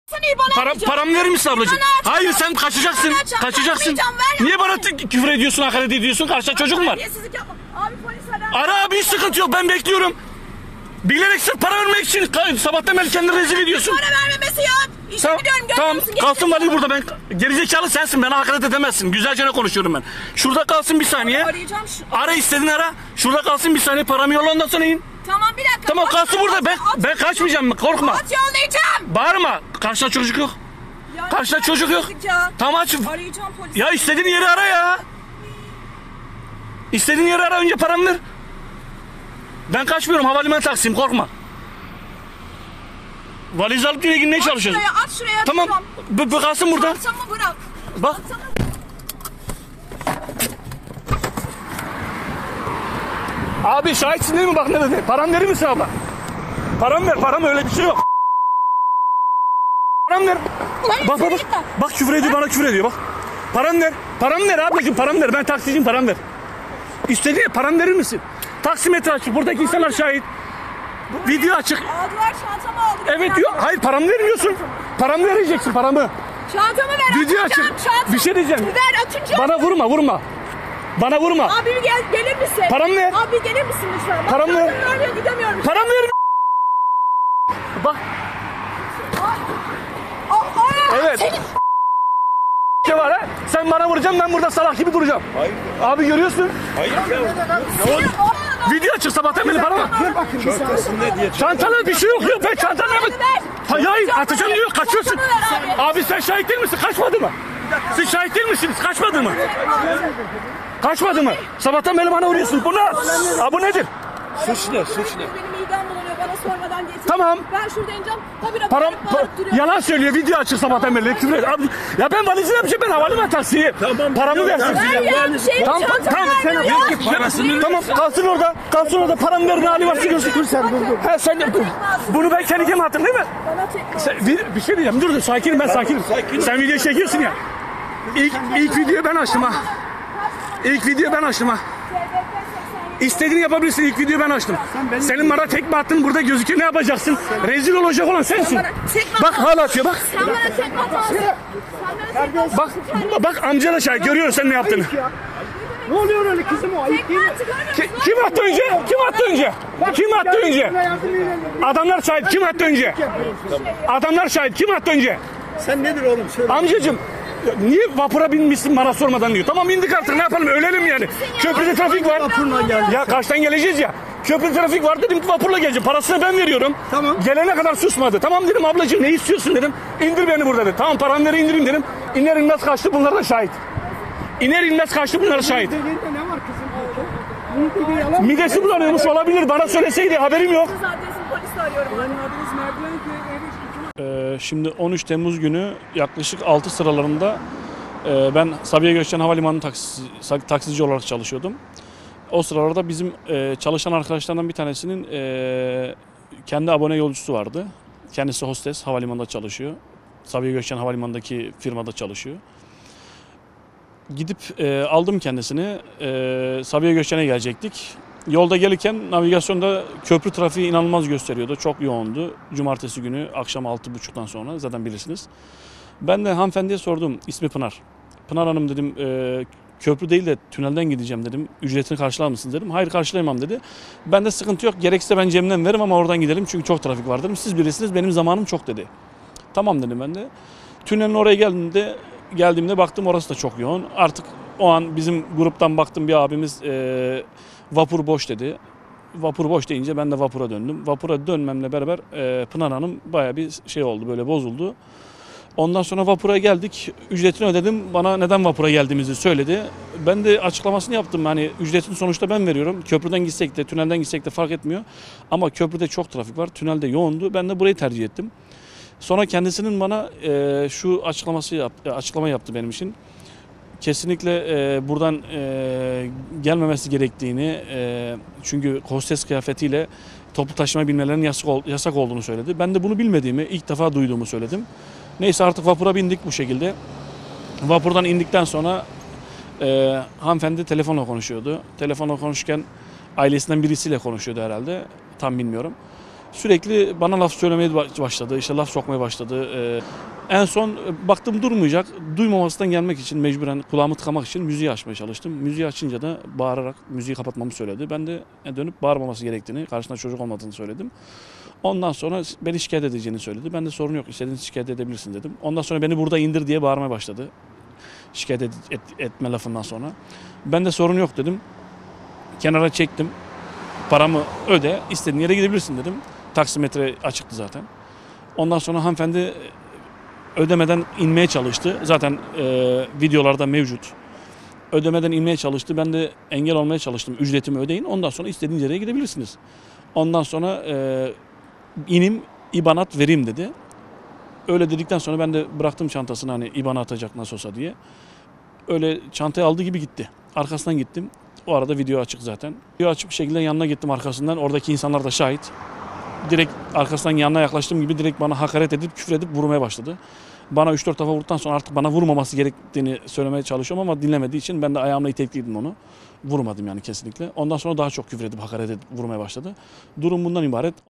Üsküdar’da taksici ile hostes arasındaki tartışma kamerada
Güngören’den Sabiha Gökçen Havalimanı’na gitmek için taksiye binen bir hostes, ücret ödememek için çeşitli bahaneler üretti. Hostes ile taksi şoförü arasında ücret ödememe nedeniyle tartışma çıktı. İkili arasındaki tartışma ise taksicinin cep telefonu kamerasına yansıdı.